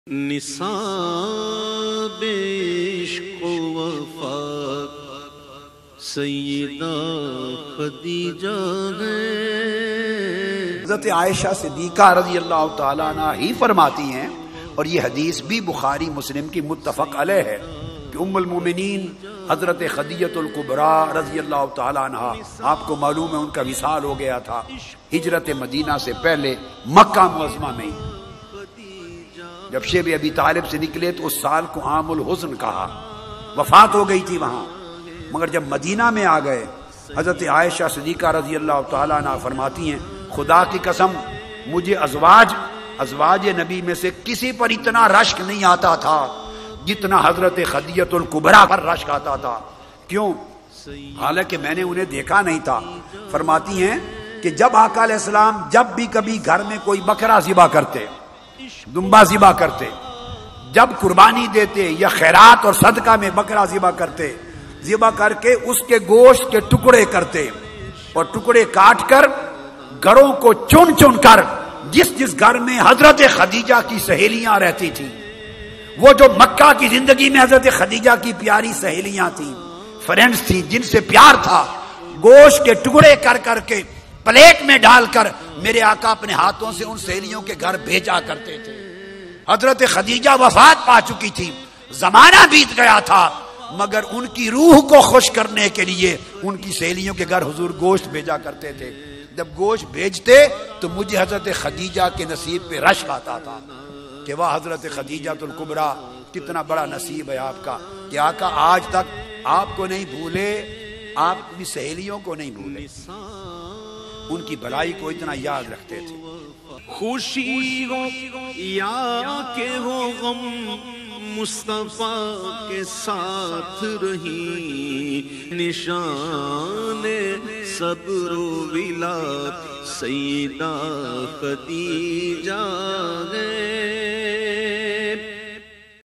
Khadīja bint Khuwaylid (may Allah be pleased with her)
Today (10th Ramadan) marks the day on which Khadīja bint Khuwaylid (may Allah be pleased with her) passed away